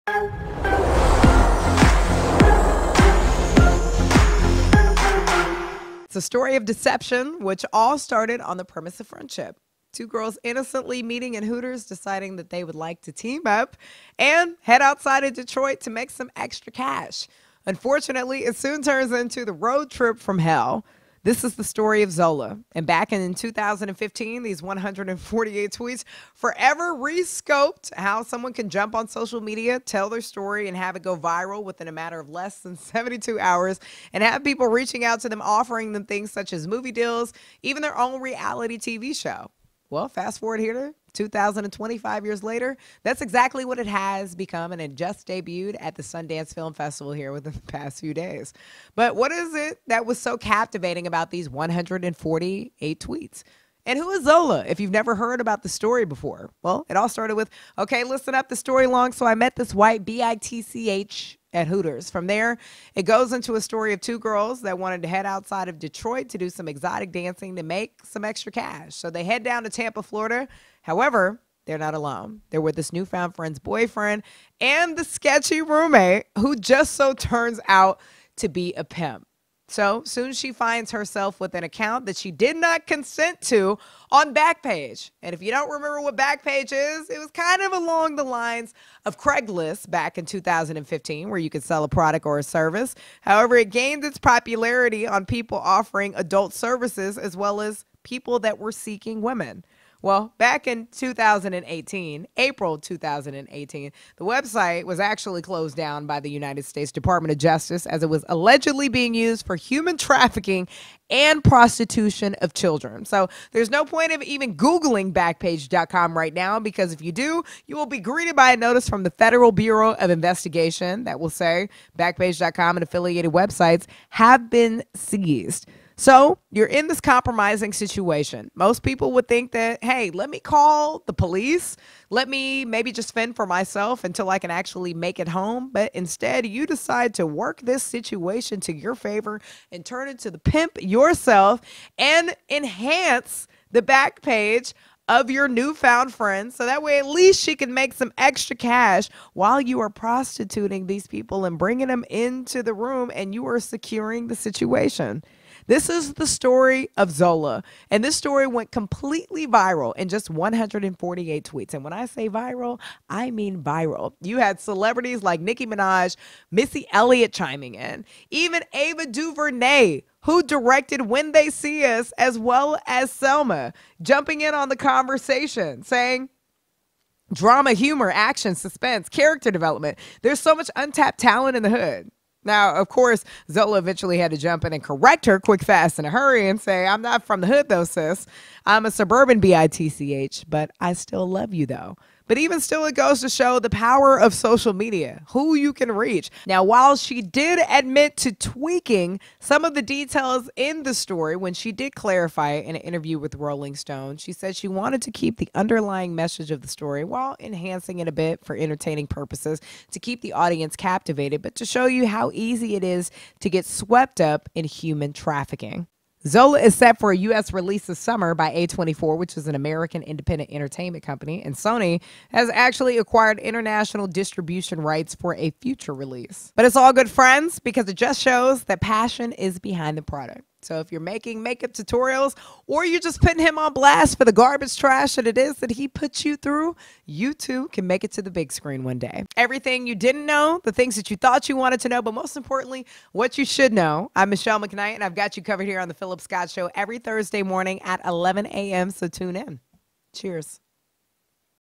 Zola The Stripper's Viral Tweet Turned Into Sundance Film Festival Movie
Zola, which premiered at the Sundance Film Festival, is as compelling as its Twitter source material. Adapted from King’s thread and directed by Janicza Bravo, it’s a stylish dramatization of the dramatization, polishing a social media narrative into something neater and a little more conventionally dramatic, but also bringing it to life with an excellent cast and vivid look.

Hosted by Phillip Scott